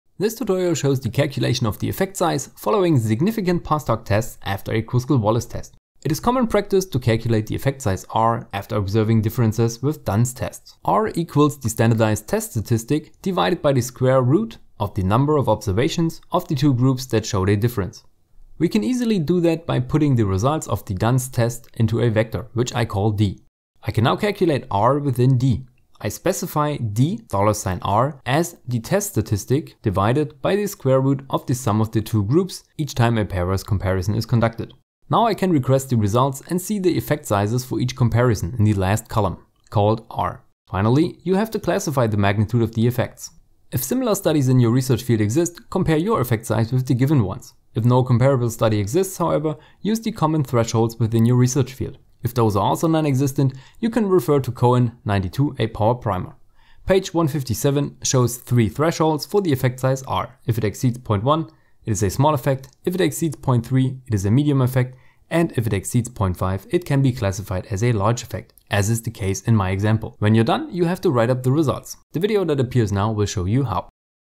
Effect size for post-hoc-tests of the Kruskal-Wallis-test in R
After significant post-hoc-tests for the Kruskal Wallis-test in R an effect size calculation is highly recommended. Therefore, the effect size r needs to be calculated.

The effect size r is the test statistic divided by the squareroot of the sum of the two groups each time a pairwise comparison is conducted:
r = z/sqrt(n1+n2)

The magnitude of the effect size is classified with comparable studies or research field specific thresholds. If neither exist, you can refer to Cohen (1992): A Power Primer on page 157.

📚 Literature:
- Cohen, J. (1992): Quantitative methods in psychology: A power primer. Psychological bulletin, pp. 155-159.
- Wasserstein, R. L., & Lazar, N. A. (2016). The ASA statement on p-values: context, process, and purpose. The American Statistician, 70(2), 129-133.

The tutorial is based on my video: Post hoc tests for the Kruskal Wallis-test in R I use Dunn's tests with the dunn_test-function of the rstatix package within it.

⏰ Timestamps:
0:00 Introduction and prerequisites
0:18 Calculating r
0:40 Classifying r

If you have any questions or suggestions regarding Effect size for post-hoc-tests of the Kruskal-Wallis-test in R, please use the comment function. Thumbs up or down to decide if you found the video helpful.